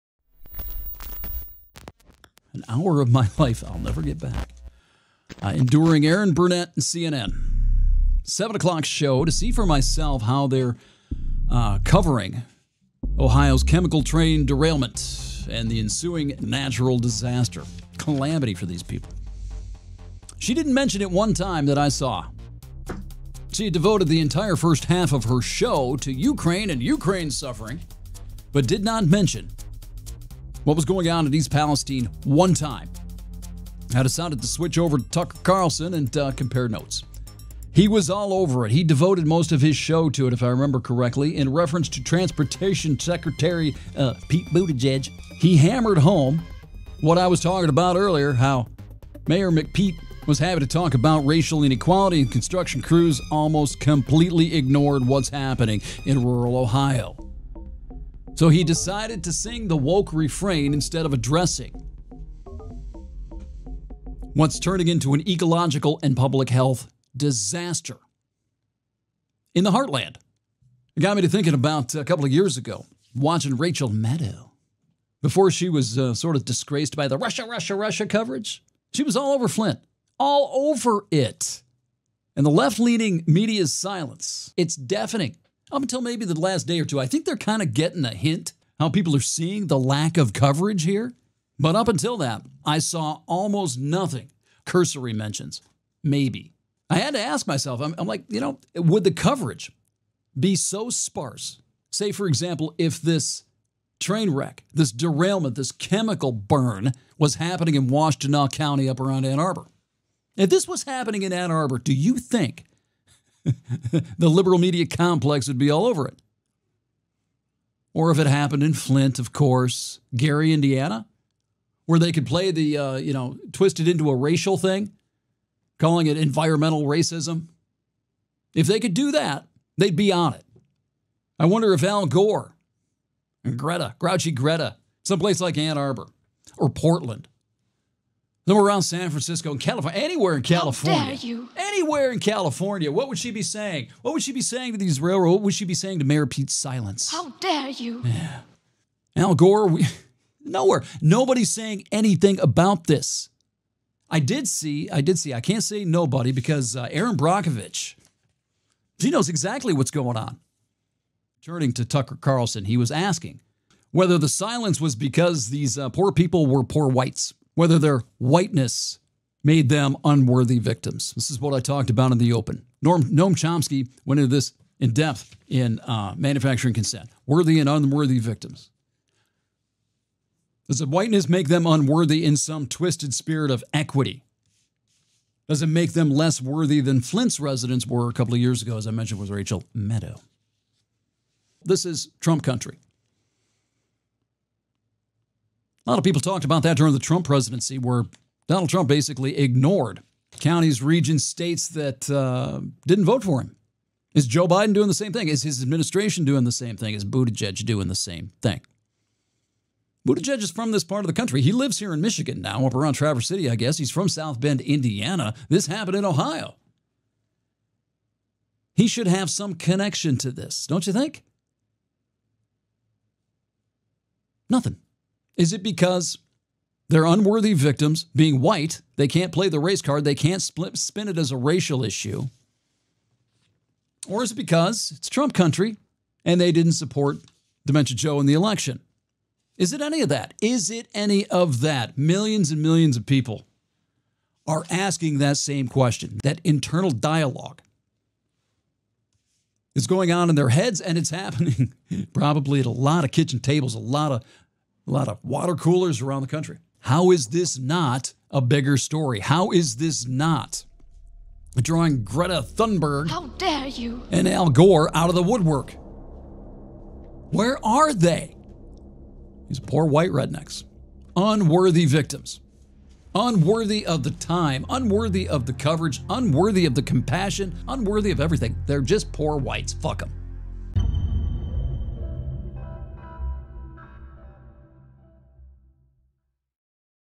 "Let The White Trash Burn!"
And East Palestine screams with millions of others: "How dare you!"

From episode 116: Guns, Balloons, Trains, Unworthy Victims